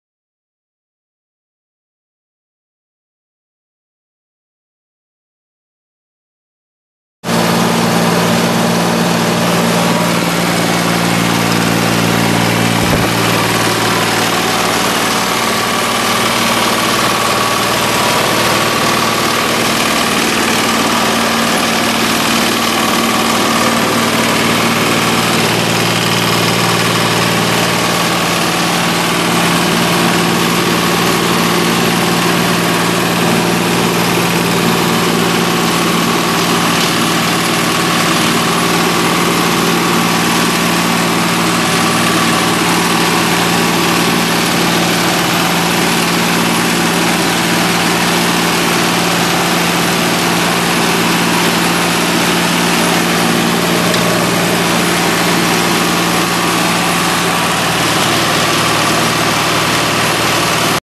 Diamond Products CC6560 concrete saw for sale | no-reserve Internet auction March 30, 2017
Diamond Products CC6560 concrete saw
599 hours on meter
Deutz three cylinder diesel engine
Hydrostatic transmission
Serial 1299567